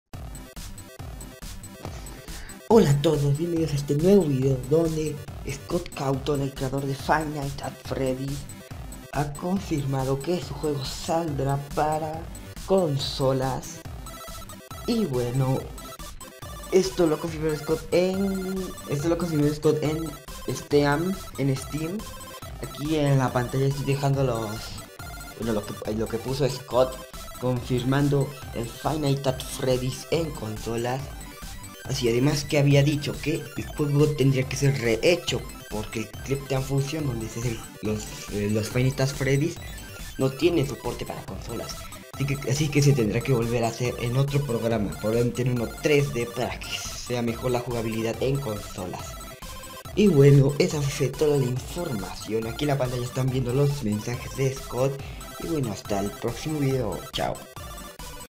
FNAF en consolas confirmado
Scott cawthon el creador de FNAF a confirmado que saldra para consolas y segun el el juego sera re echo talves en 3D para que se adapte mas la jugabilidad a controles de consolas